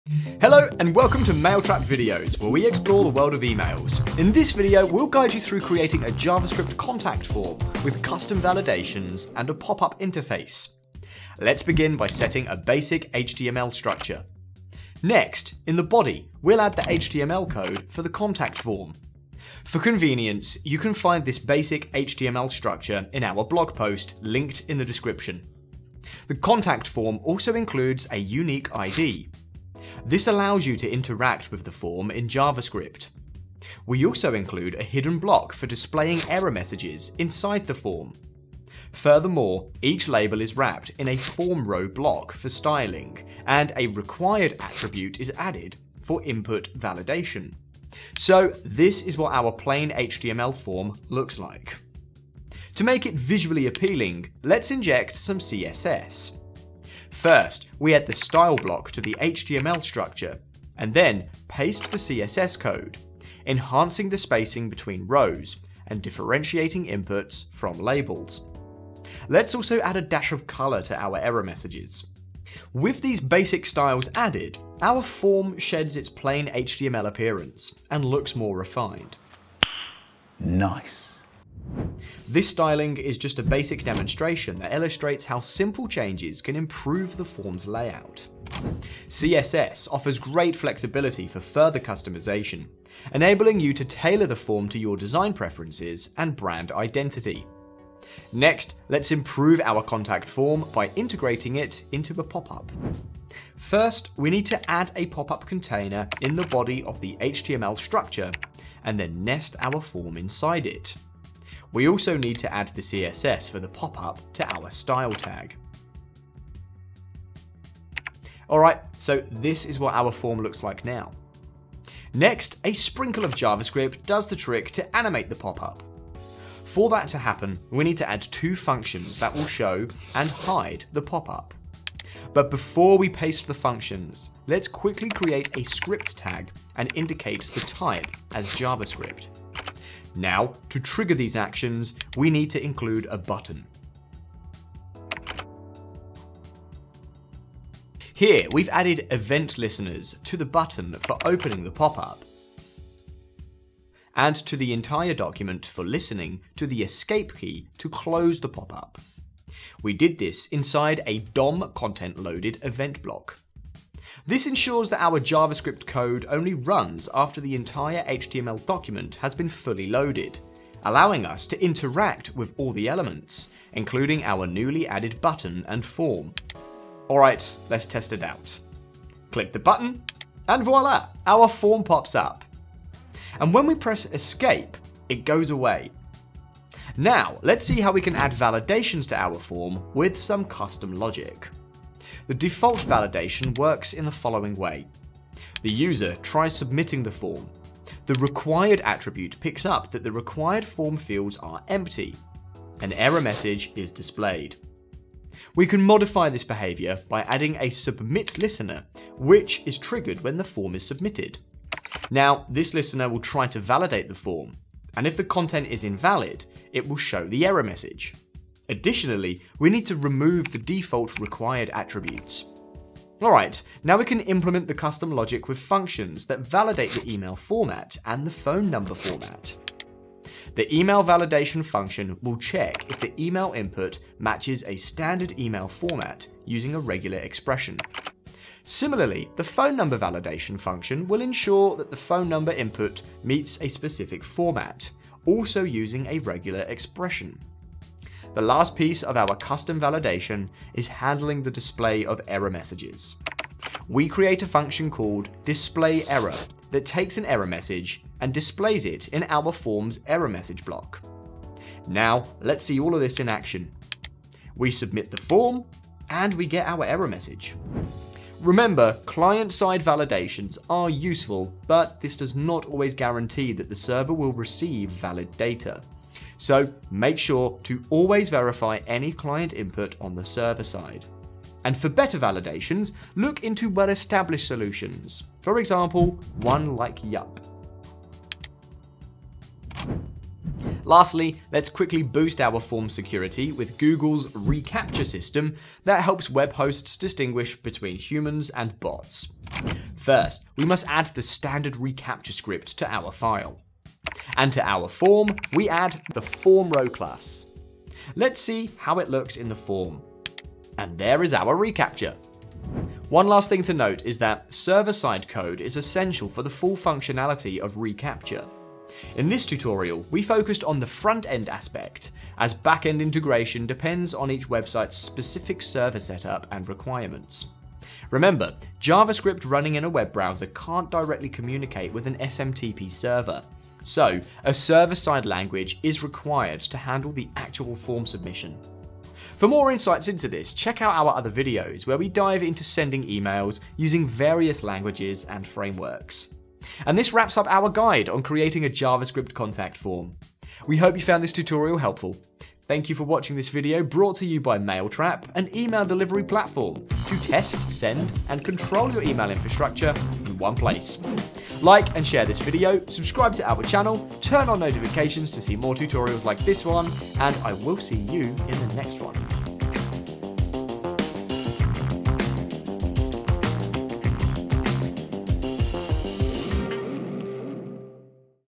JavaScript Contact Form in 2026
Discover how to create an effective HTML contact form to send email with JavaScript. In this Mailtrap tutorial we'll guide you through each step o54f establishing a basic JavaScript contact form, transforming it into an engaging pop-up, implementing crucial data validations, and enhancing security with Google's reCAPTCHA.

✅ Setting Up Basic JavaScript Contact Form (00:11)
✅ Making a Pop-up Contact Form (01:25)
✅ Contact Form Data Validation (03:06)
✅ Adding reCaptcha to the Contact Form (04:50)